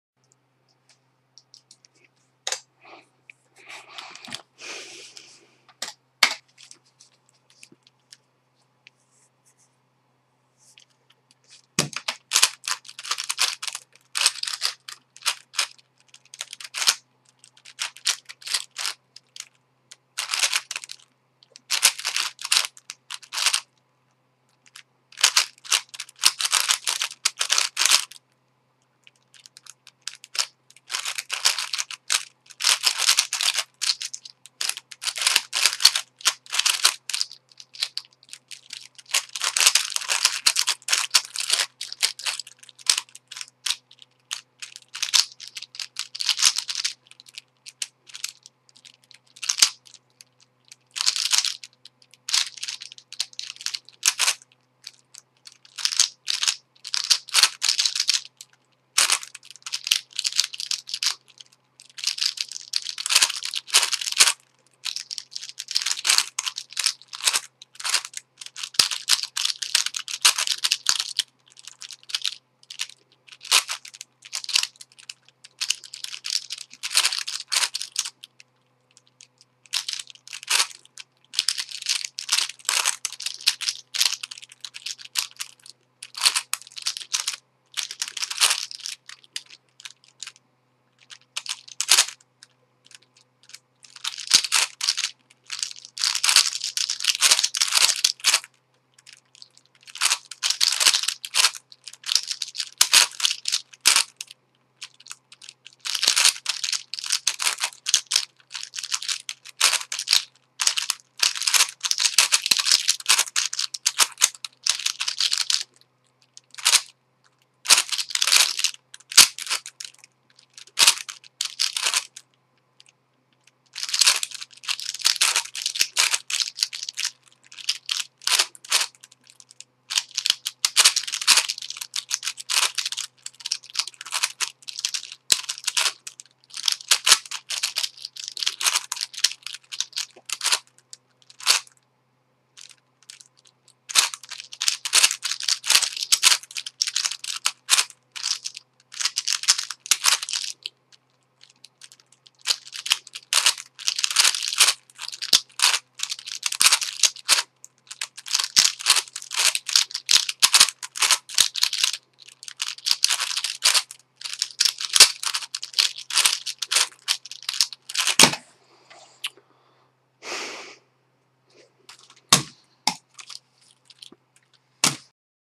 witeden 3x3x9 (regular) in 2:36.17
Not really happy is I usually do sub 2:15 easily, but it's still an okay time, since I don't practice.

The system used is an adaptation of the k335 to the 3x3x9, nothing more and nothing less.

Lockups and the white plastic actually makes this puzzle hard to see for me, since I have a distrophy similar to colorblind, so black blastic with Orange instead of pink would have been WAY better to me.

Happy new year, anyway!